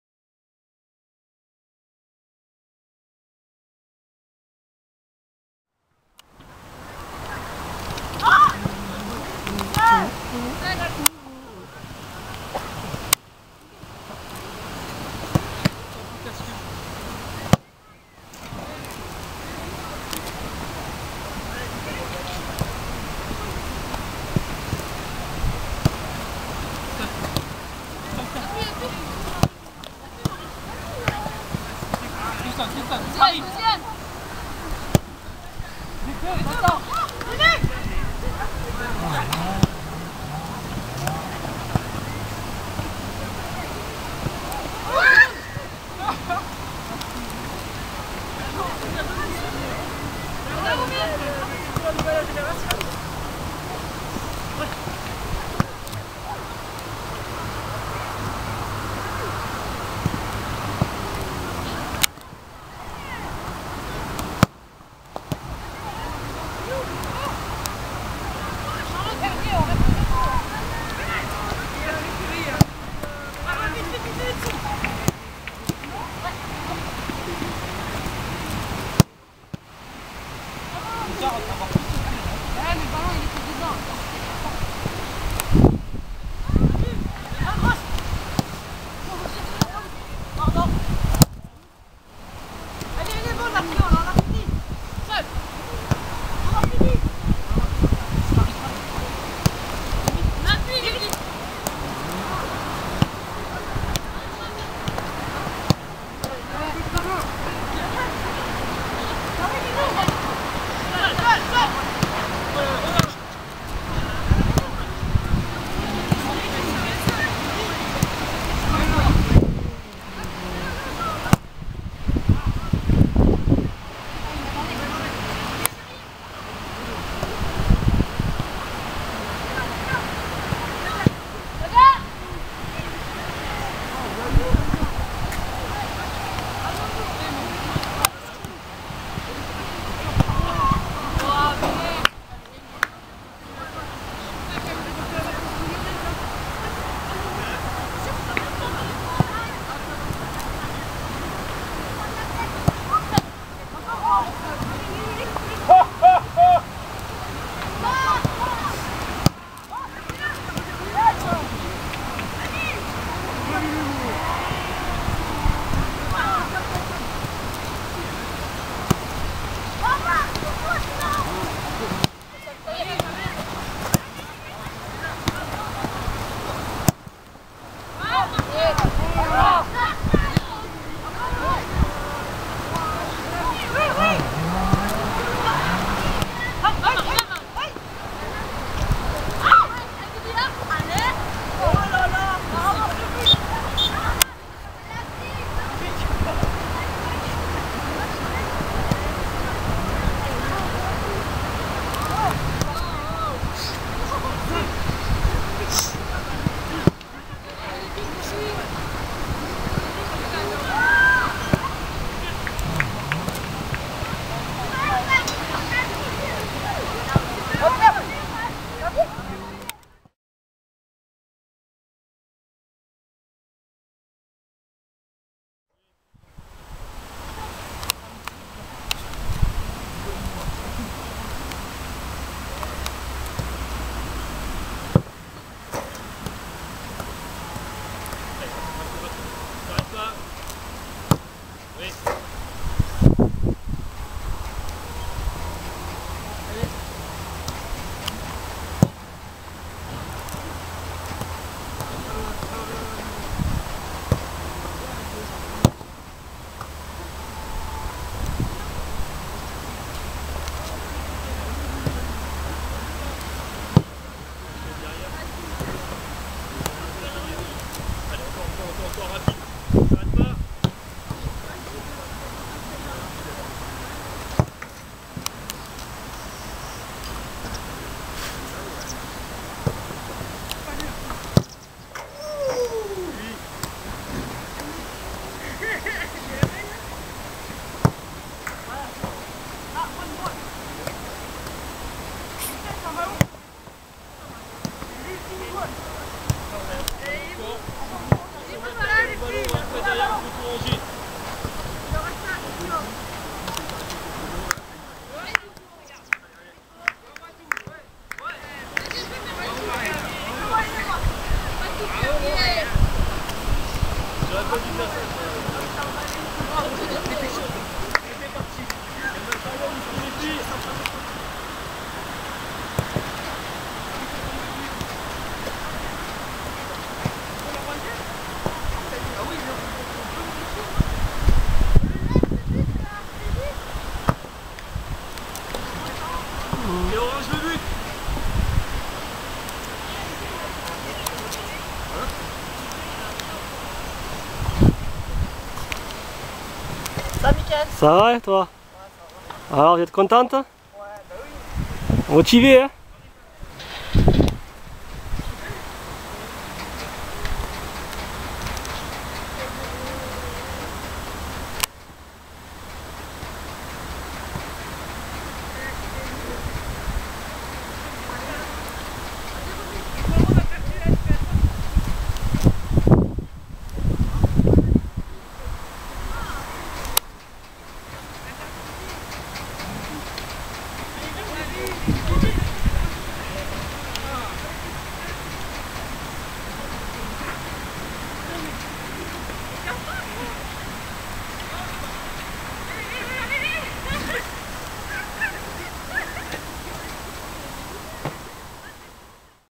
2013/05/23 & 24 MHSC WOMEN
Training before last season game agains't ASSE.
Game resulted by a draw somes days after.